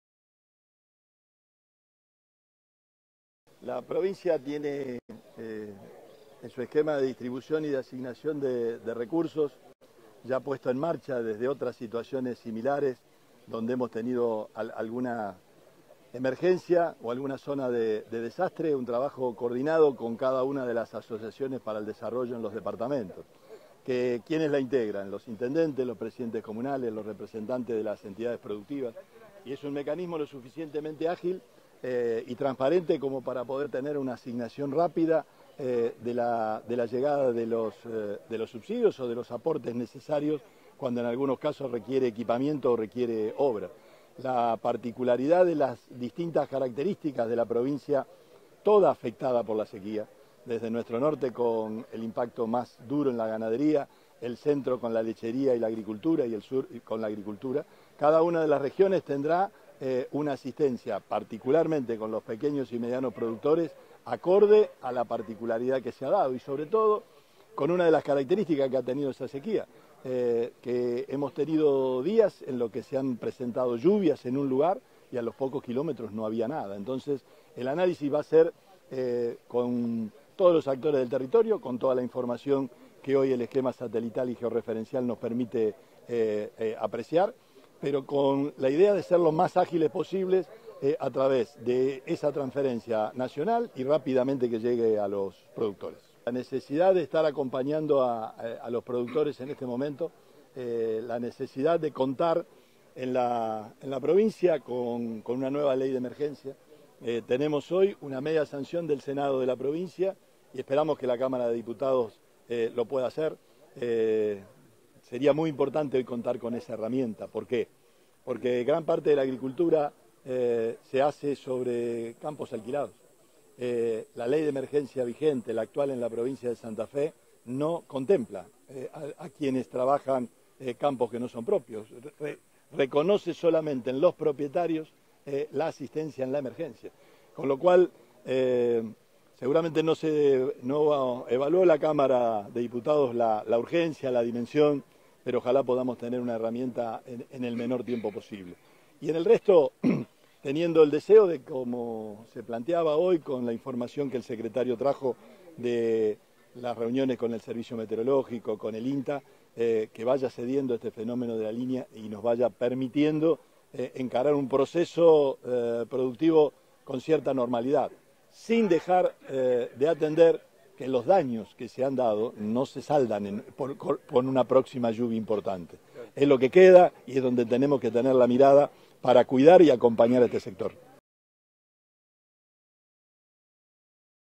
EMERGENCIA AGROPECUARIA POR LA SEQUÍA: NACIÓN DESTINARÁ $1.300 MILLONES PARA SANTA FE
El gobernador Omar Perotti y el secretario de Agricultura, Ganadería y Pesca de la Nación, Juan José Bahillo, se reunieron con productores del norte provincial afectados por la sequía, en la Estancia Las Gamas.